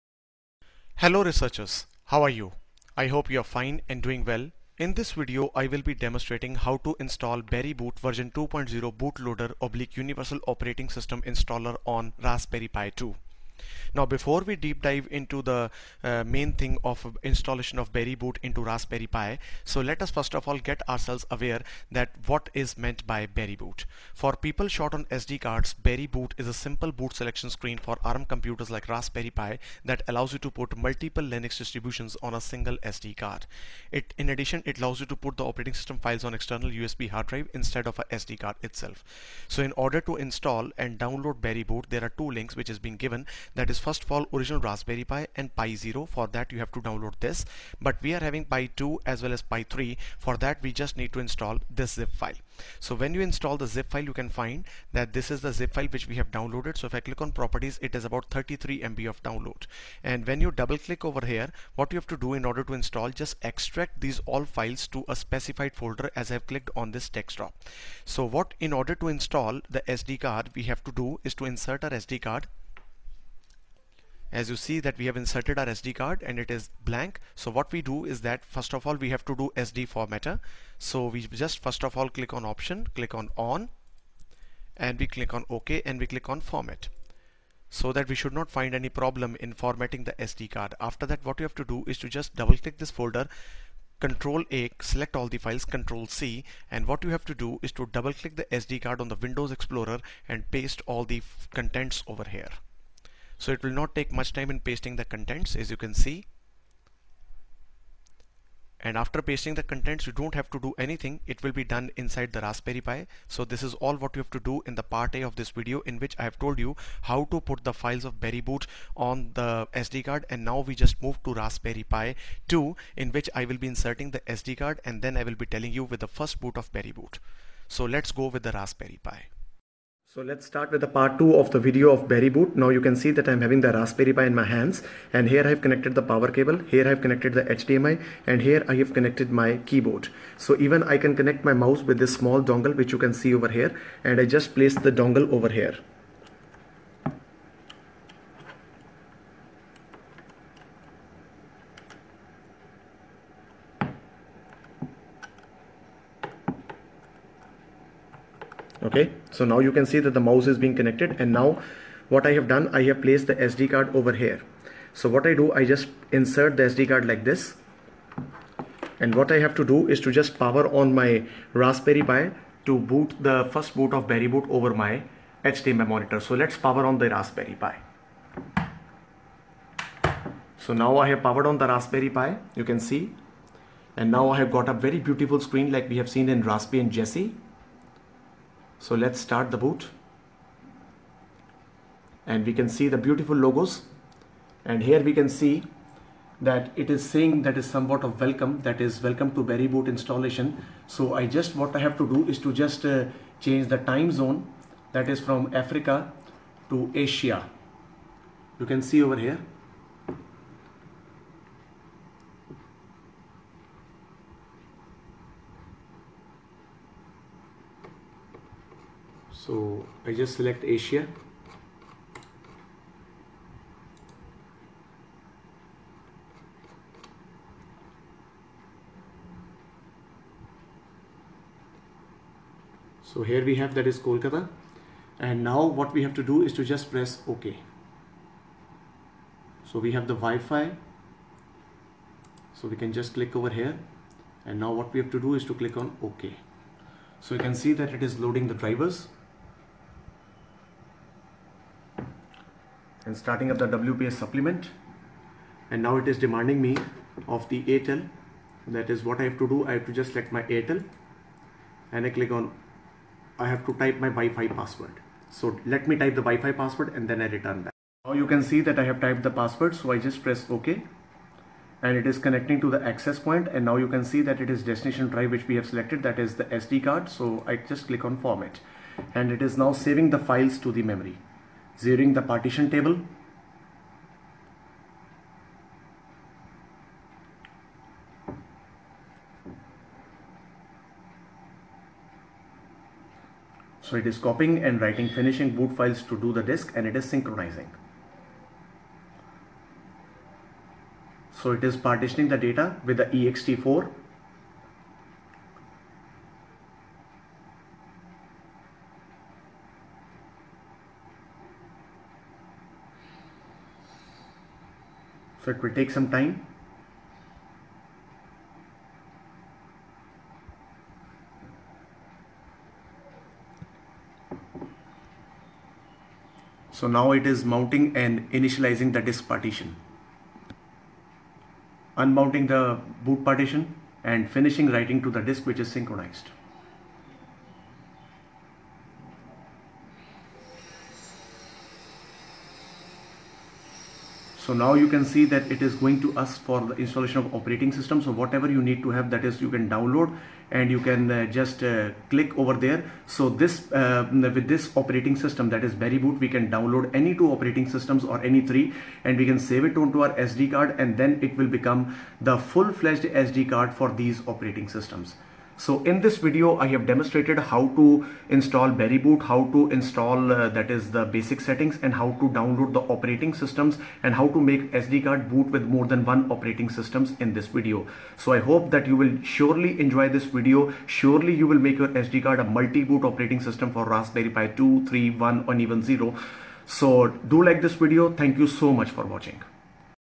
How to MultiBoot Raspberry Pi 2 via BerryBoot
In this Video, Detailed demonstration is being done regarding Installations of MultiBoot in Raspberry Pi using Berry Boot Operating System...